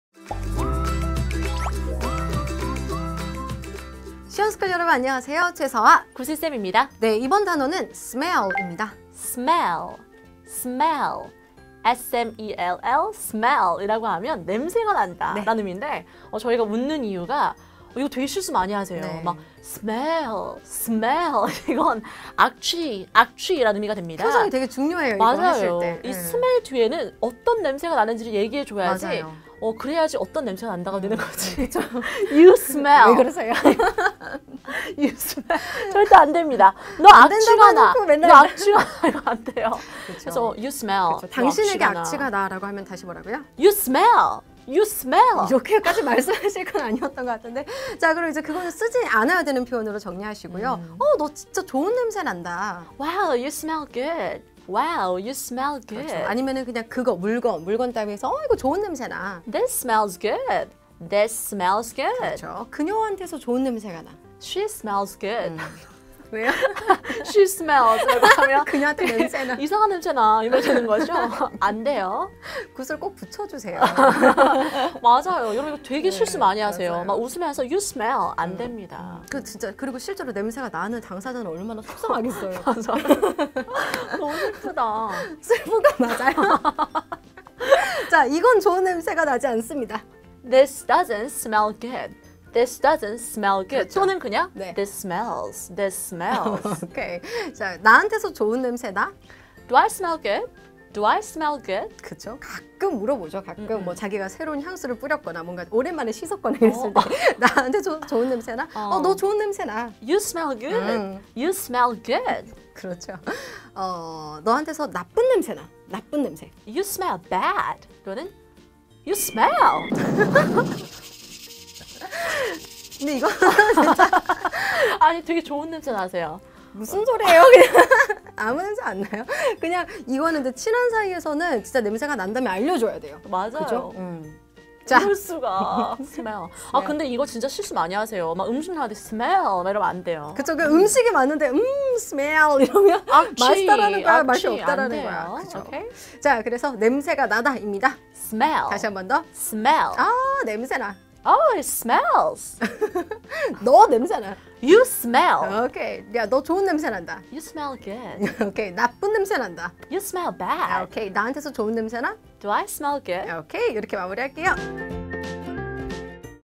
기적의 영단어 탈출기 with 최서아·구슬쌤 452강 by [시원스쿨]
영어공부는 하고 싶은데 도저히 갈피를 못 잡겠다고?
영어 왕초보들을 위해 시원스쿨이 준비했습니다.
영어왕초보 X 100강, 기적의 변화는 시원스쿨에서!

☑️ TODAY'S LEARNING
최서아·구슬 강사의 기적의 영단어 탈출기 452강